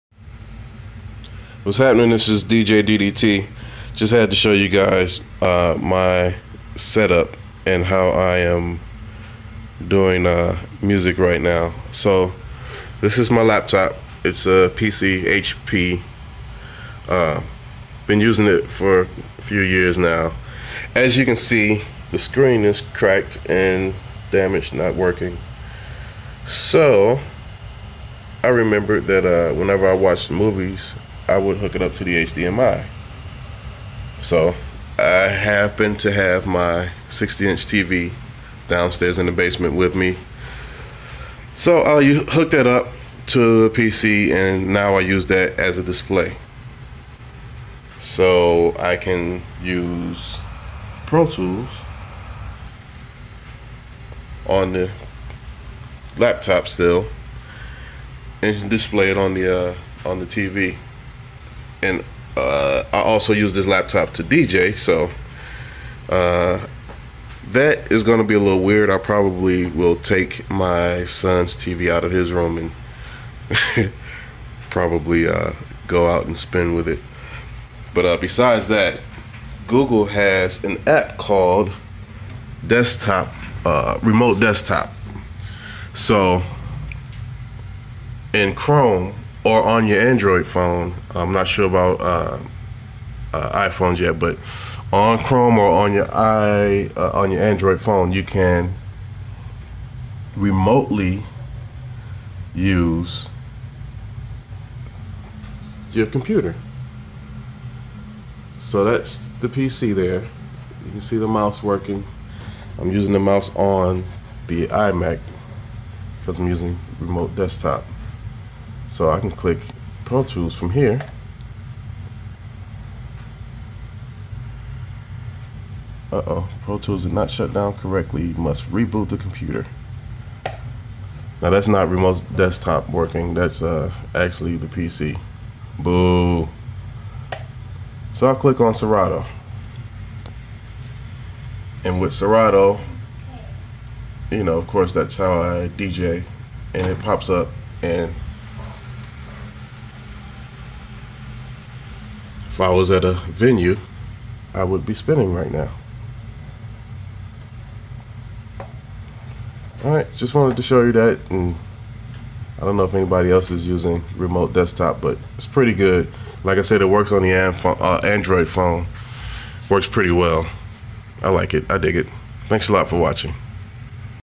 Broken Screen, HDMI, Google Remote Desktop
Just a show of how I'm using my laptop, since the screen was damaged.